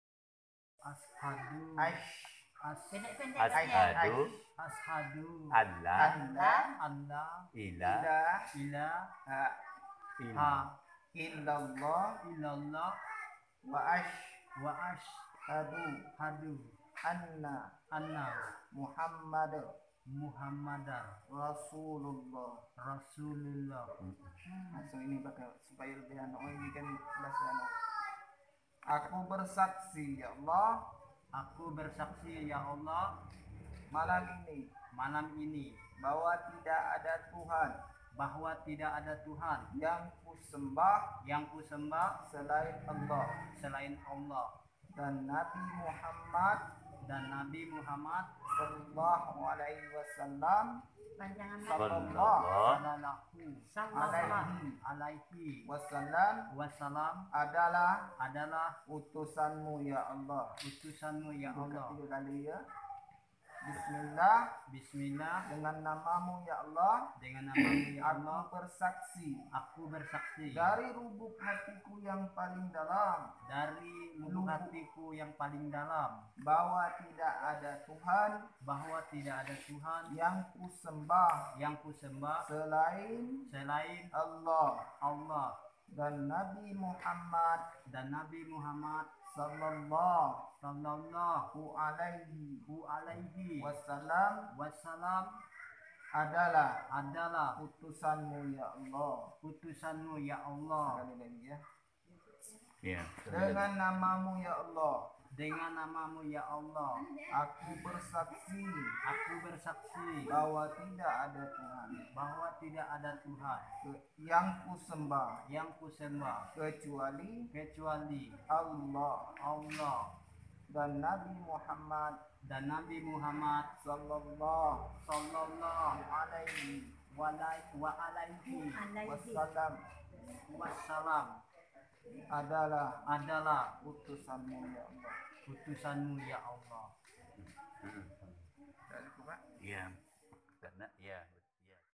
Hidayah-Nya Datang di malam hari
Beginilah suasana Pengislaman malam kemarin meski lampu masih belum ada disurau tapi proses Pengislaman tetap berjalan dengan lancar